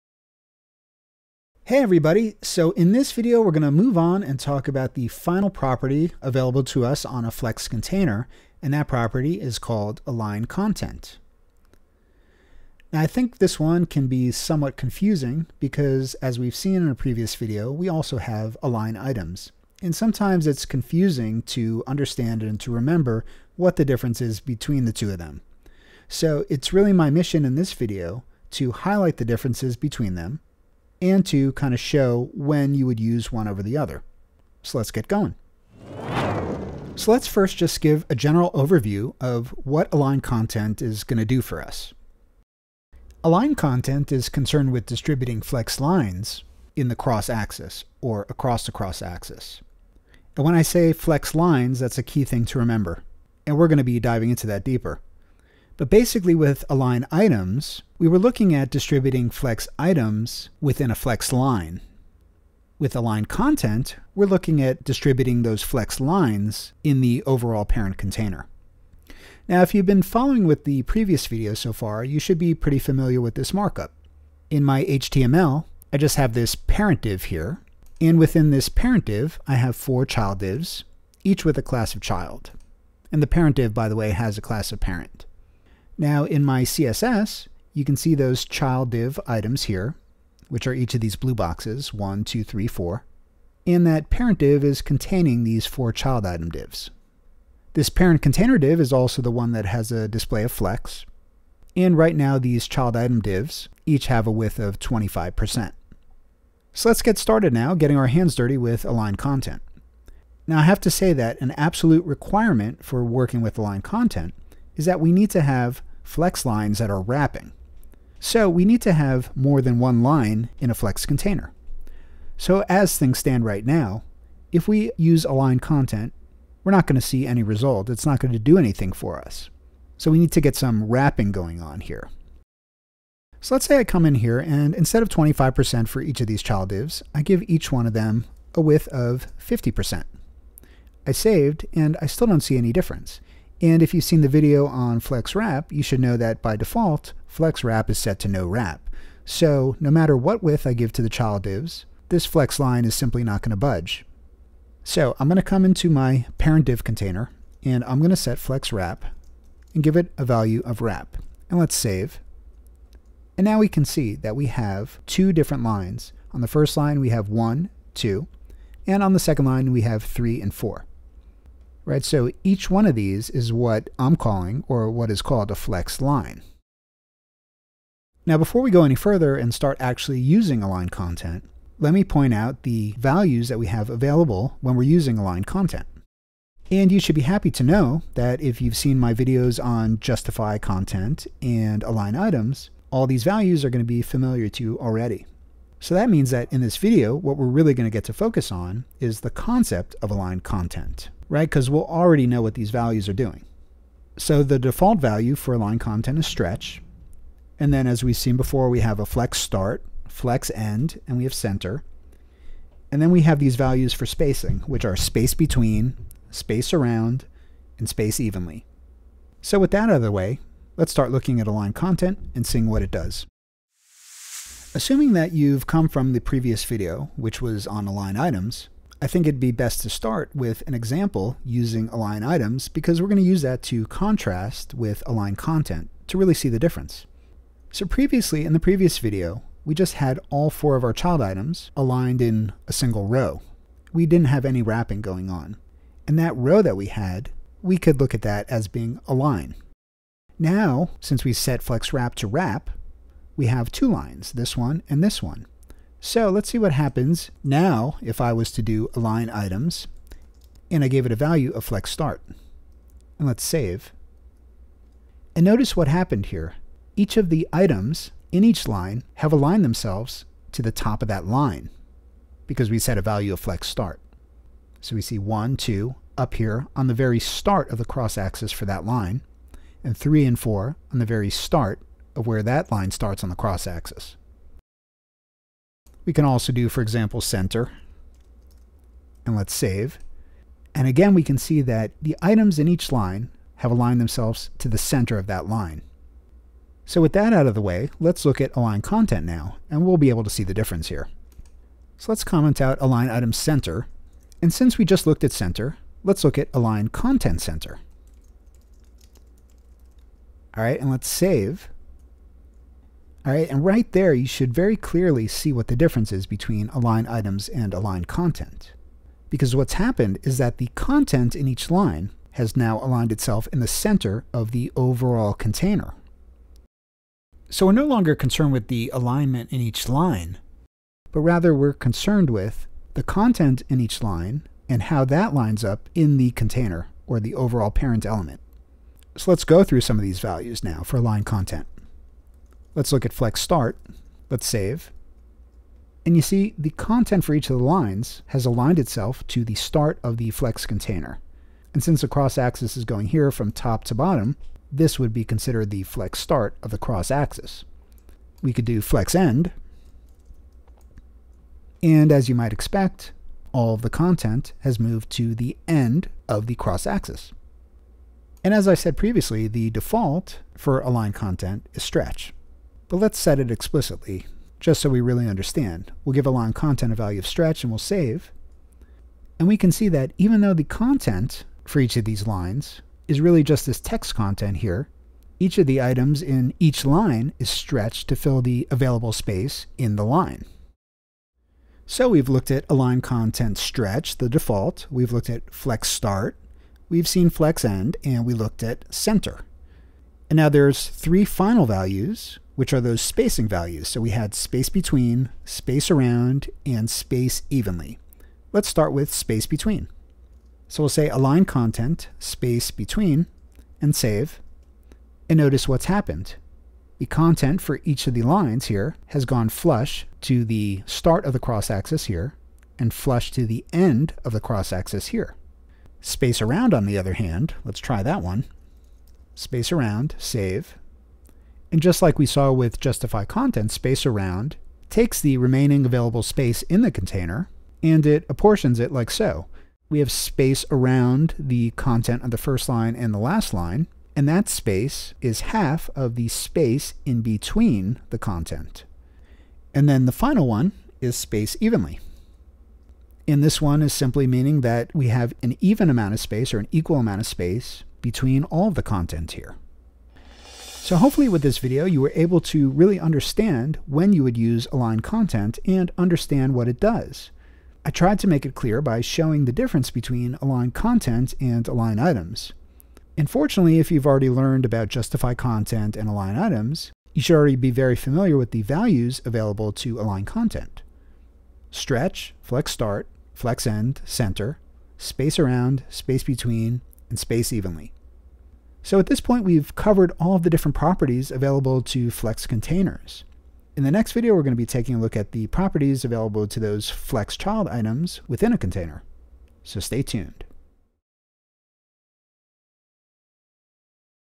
Spacing Elements with CSS Flexbox: A Guide to Using the Align-Content Property
CSS Flexbox Align-Content: Master the Align-Content property and understand the difference between Align-Content and Align-Items.

The Align-Content property in CSS Flexbox can be a bit confusing, but don't worry! This video will break it down and help you understand how it works and how it differs from the Align-Items property.

You'll learn about the various values you can use with the Align-Content property, including "flex-start", "flex-end", "center", "space-between", "space-around", and "stretch".

Get ready to take your CSS Flexbox skills to the next level with the Align-Content property. Created by Gregg Fine.